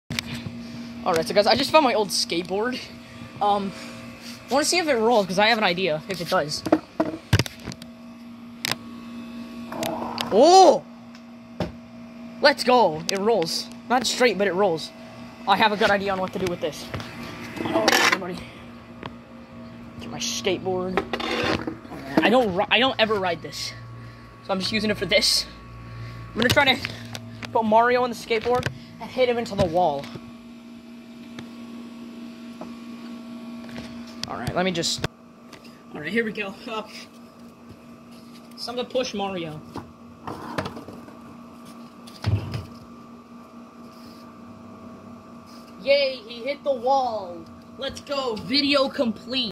super mario rides a skateboard and fucking dies
there's nothing i can say here so:

penis